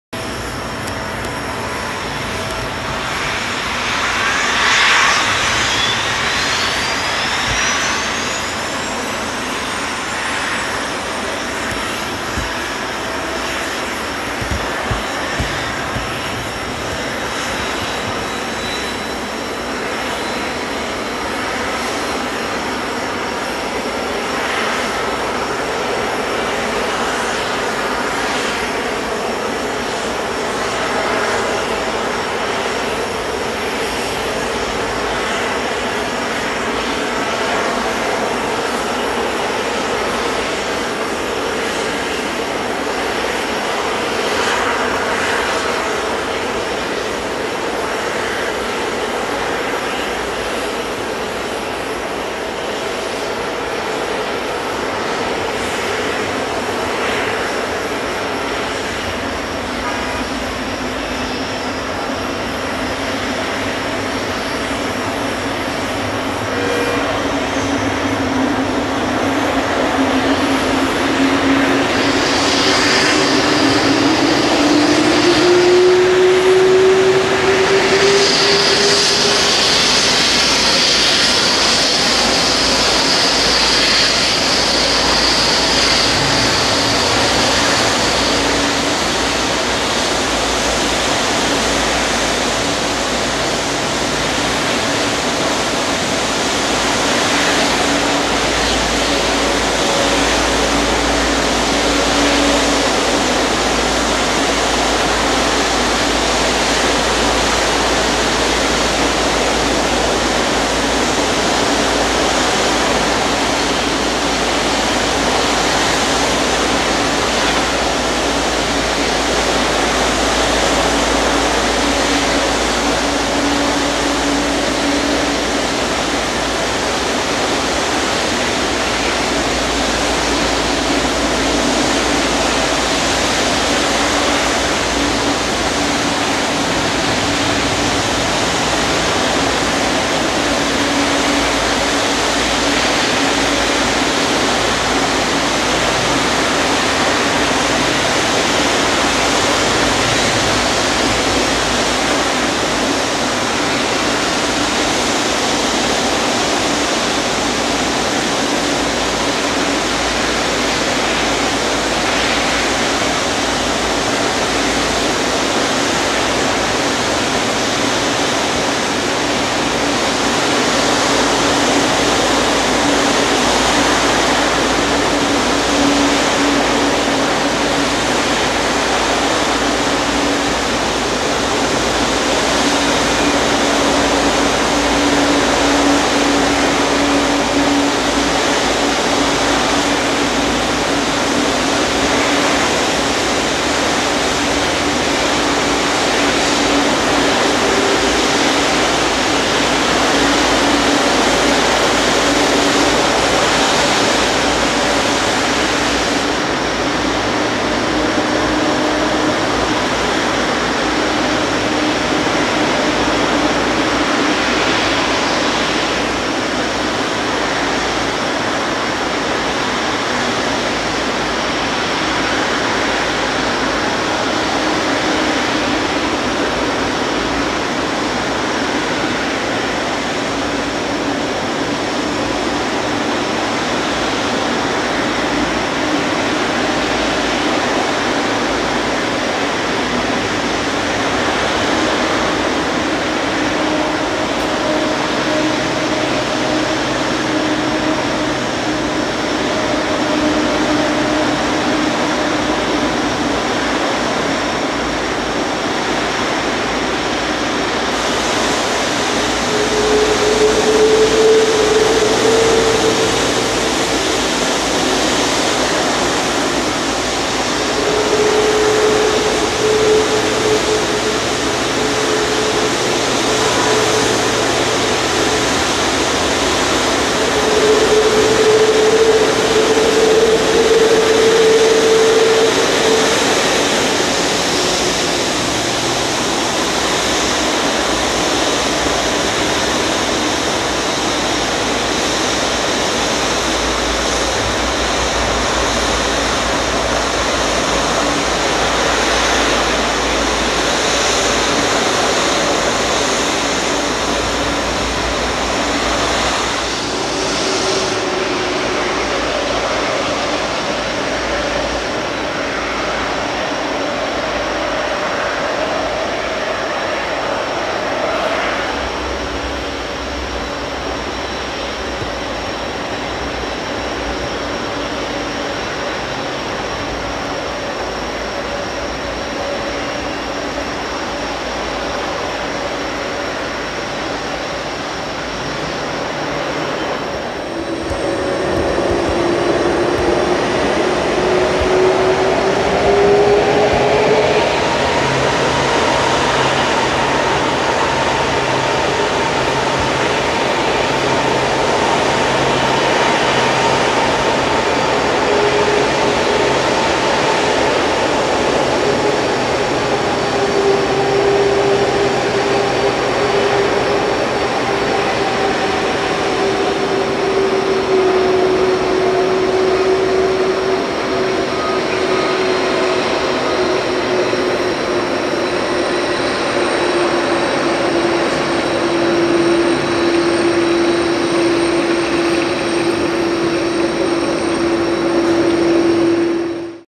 AAL Boeing 777-200ER & ANZ Boeing 767-300ER Pushback - NRT Terminal2 -
American Airlines Boeing 777-200ER
Air New Zealand Boeing 767-300ER